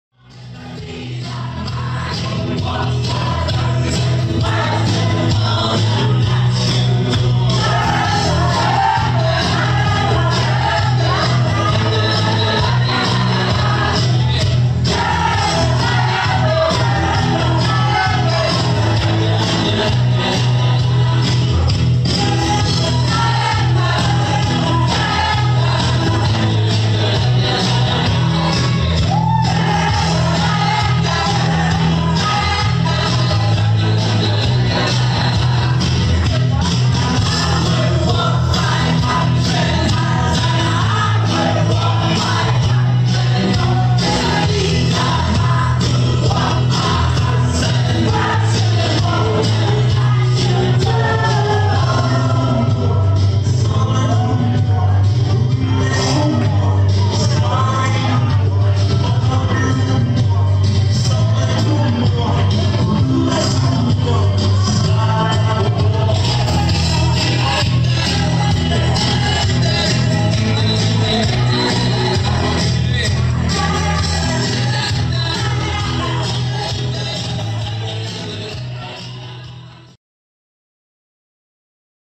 20th Jan 2024 at Falkland Palace
This was an extra add-on after a 3-dance shindig.
Bookings: ceilidh-caller-fife.co.uk